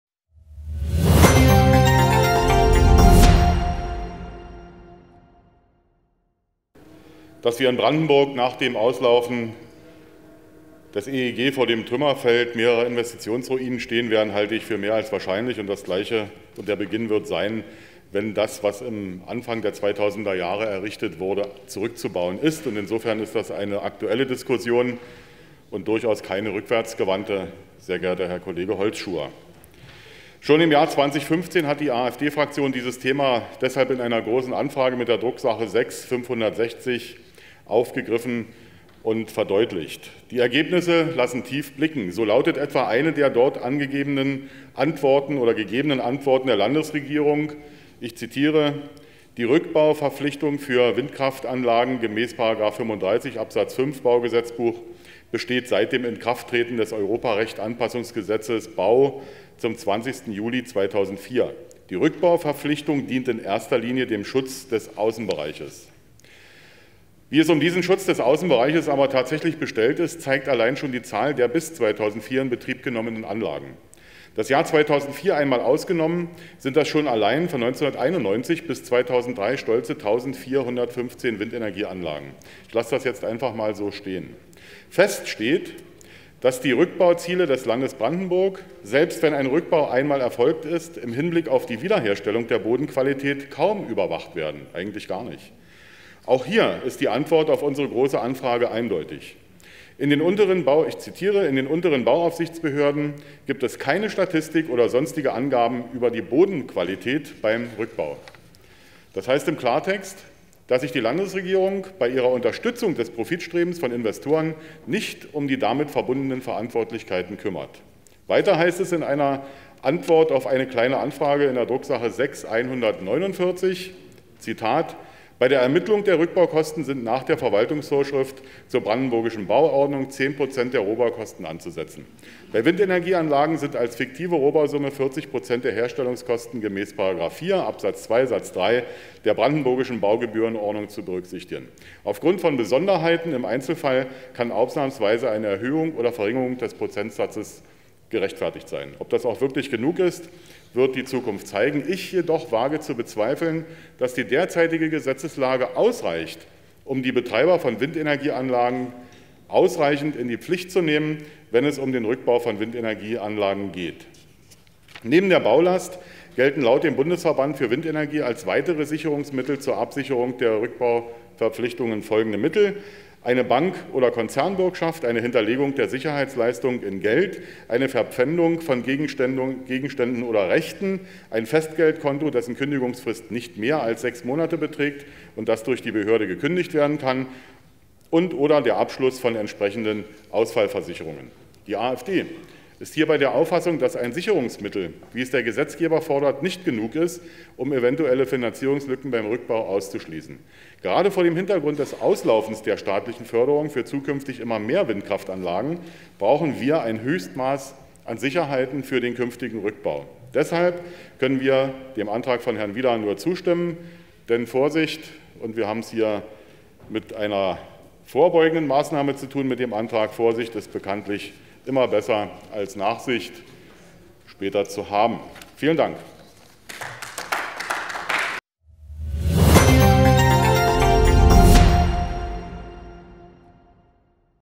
Mit Windanlagen ist viel Geld zu verdienen. Doch wer sorgt für den Rückbau? Sven Schröder (AfD)
Mit Windanlagen ist viel Geld zu verdienen. Doch wer sorgt für den Rückbau wenn die Anlagen außer Betrieb gesetzt werden? Sven Schröder spricht für die AfD